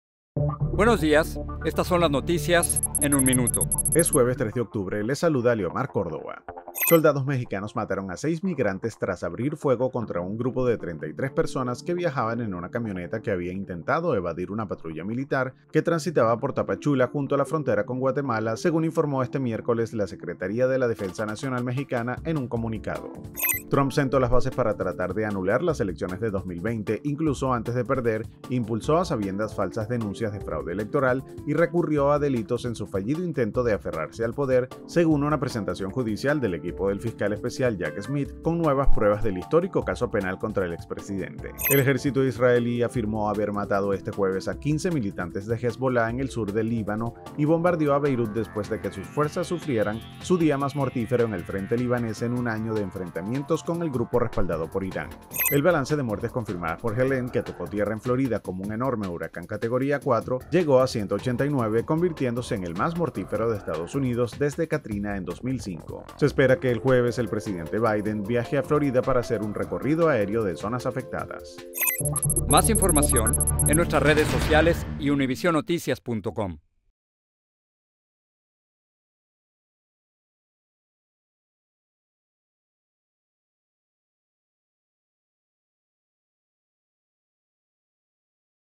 En un minuto: Soldados mexicanos matan a seis migrantes en Chiapas y otros 10 resultan heridos
El equipo del fiscal especial Jack Smith presentó nuevas pruebas del histórico caso penal contra Donald Trump; Israel bombardea el centro de Beirut y Helene se convierte en el huracán más mortífero en EEUU desde Katrina. Más información en UnivisionNoticias.com.